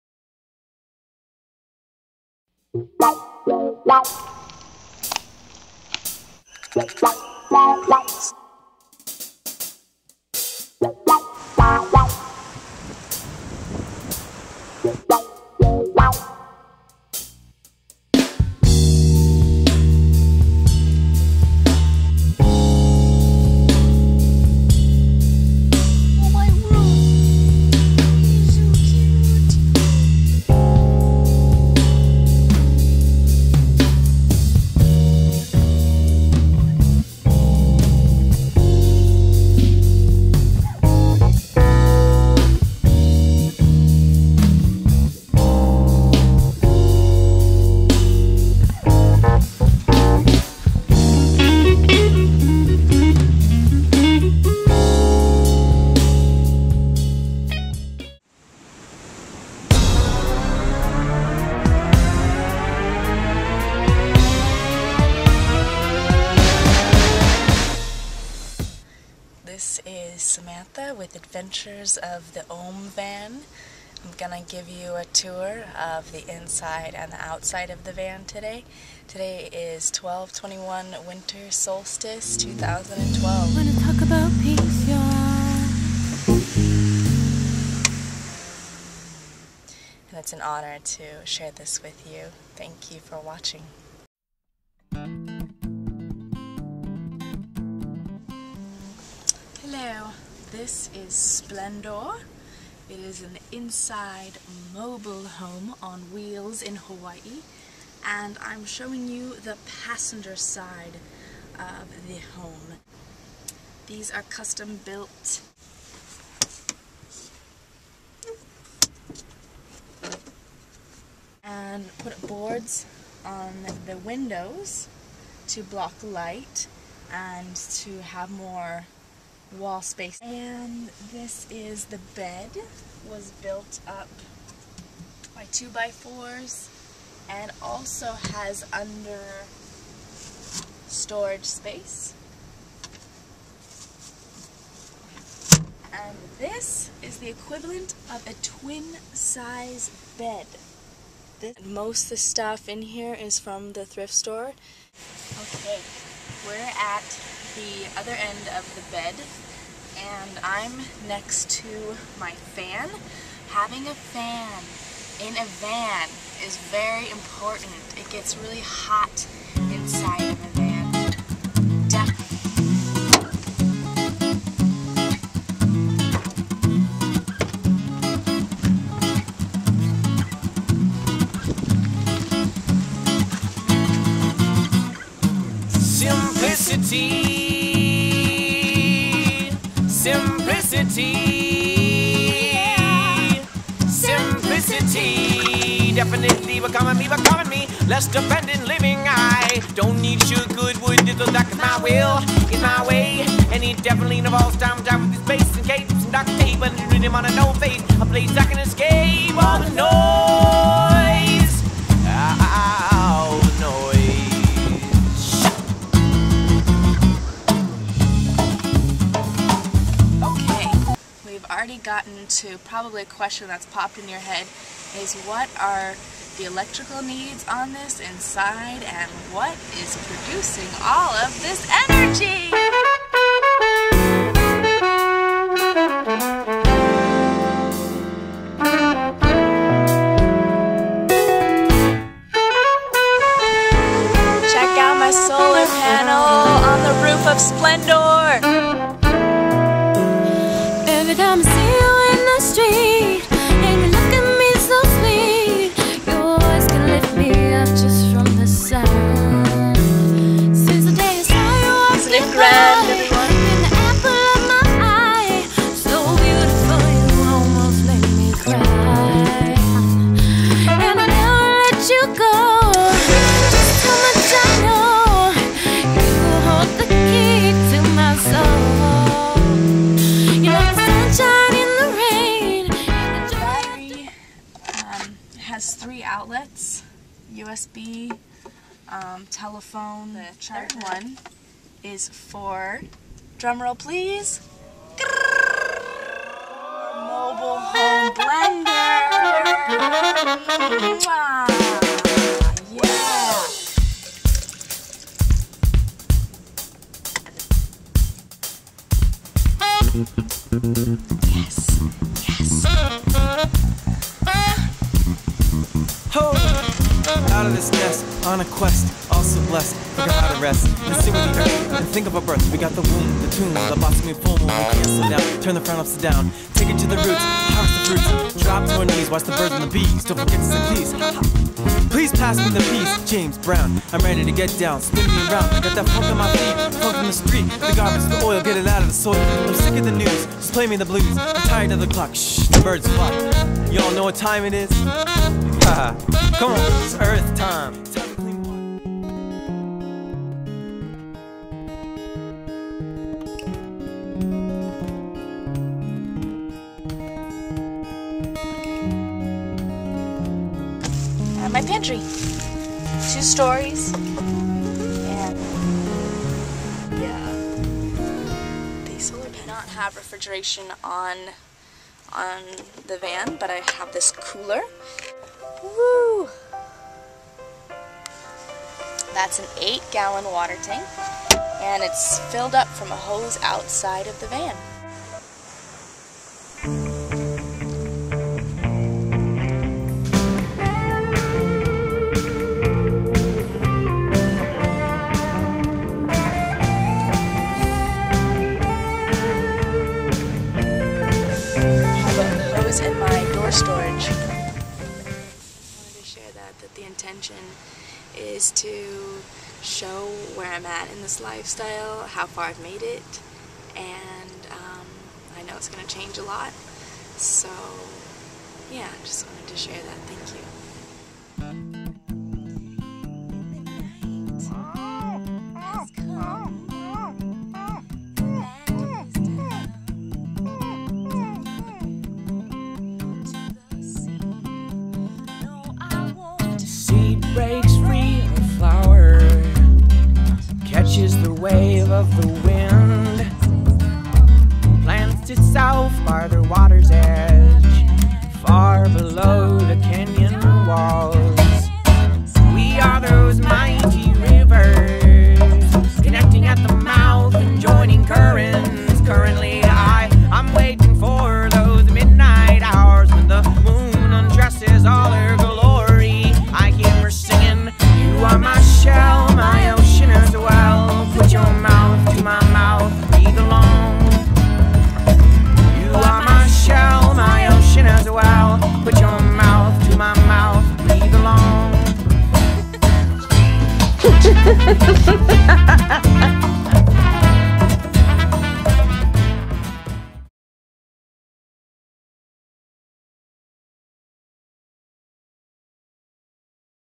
Sunshine's Van Life on the Big Island of Hawaii
Take a tour of "Splendor"-- the 15 passenger Ford van that was converted to a living hOMe, showcasing solar energy! The mobile home has a kitchen- with running water, a stove, a twin size bed, 12 volt powered fans, a blender, led lights and custom cabinets. All indoor appliances are charged by the solar panel on the roof. Check it out!